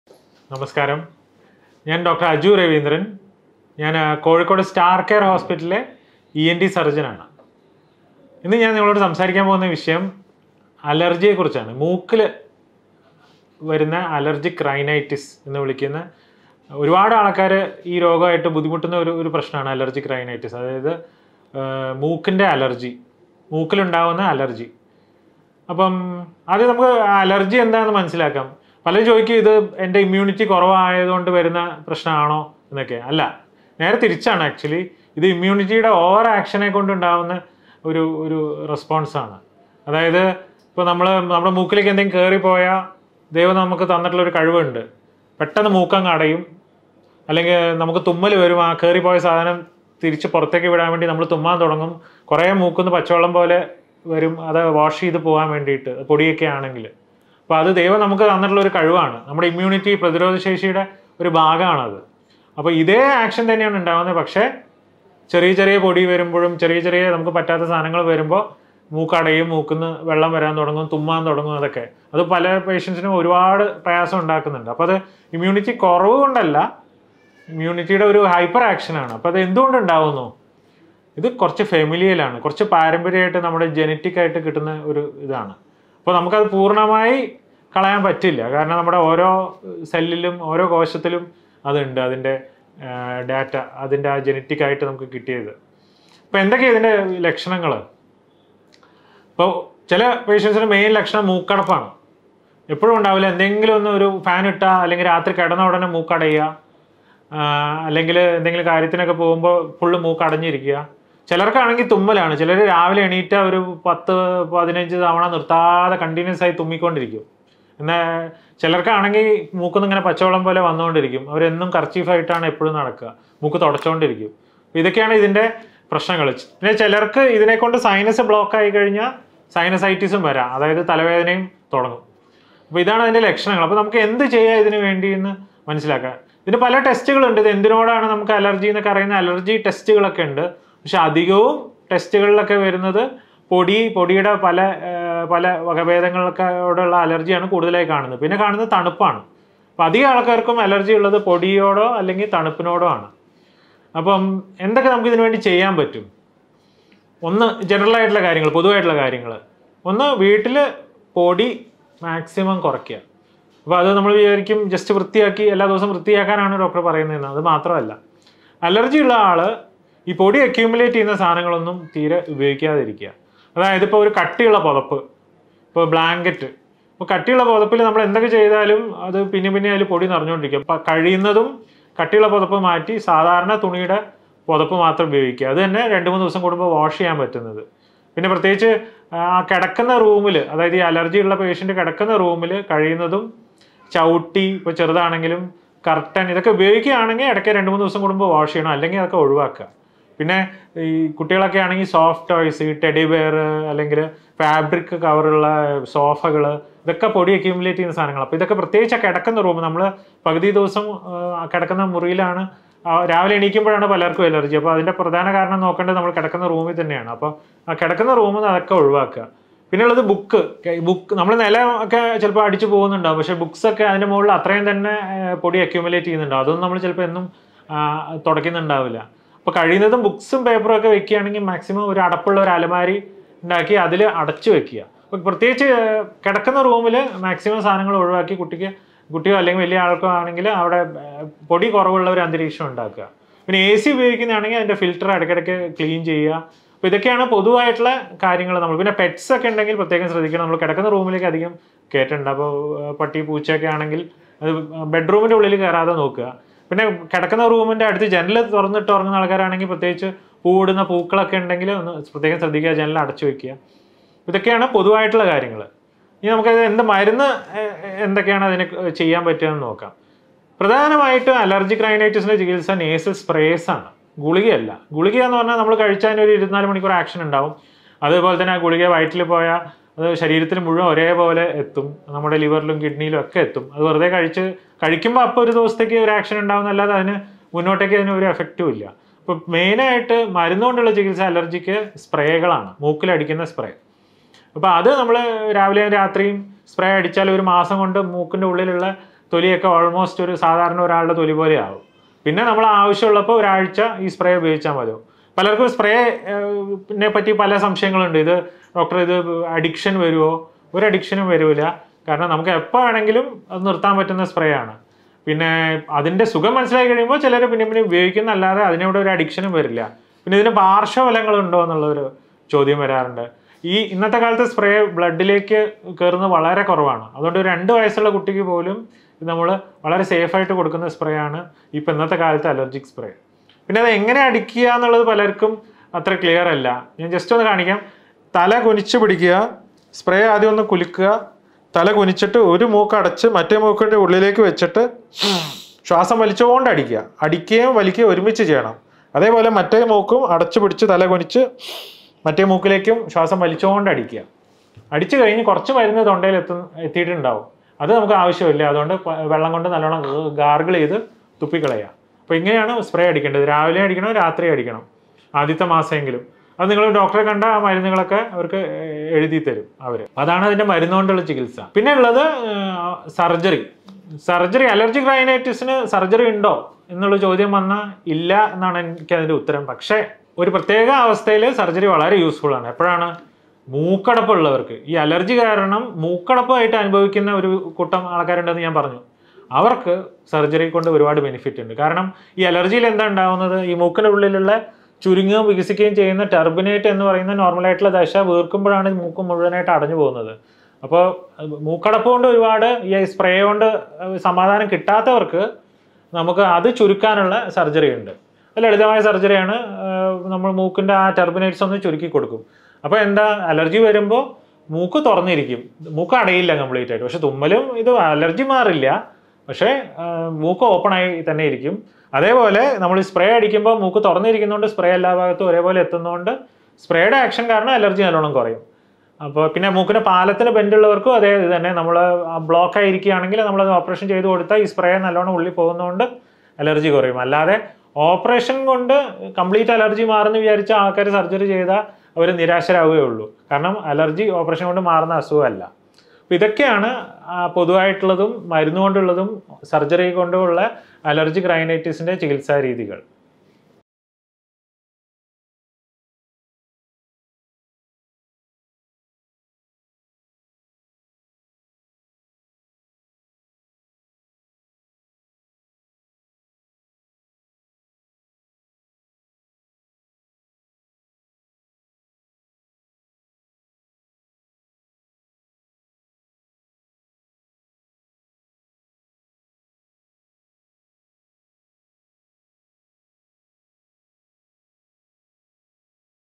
മൂക്കിലുണ്ടാവുന്ന അലർജി മാറാൻ ഇങ്ങനെ ചെയ്‌താൽ മതി | Allergic Rhinitis malayalam | Dr. Aju Ravindran
Dr. Aju Ravindran
MBBS, MS (ENT), DLO, DNB (ORL)
Sr. Consultant ENT Surgeon. Srarcare  Hospital Kozhikode..